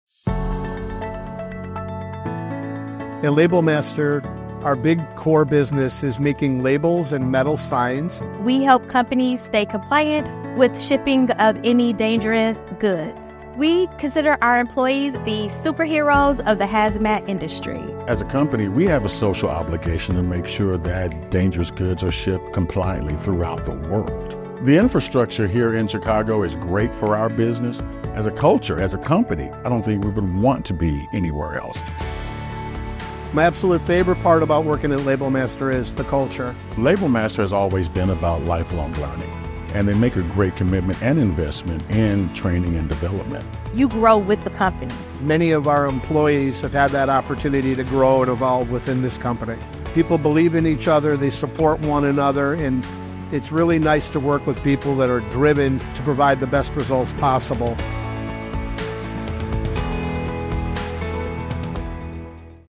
Labelmaster - Excellence in Manufacturing
Congratulations to Labelmaster - the recipient of the Excellence in Manufacturing at the 2024 Chicago Manufacturing Awards Ceremony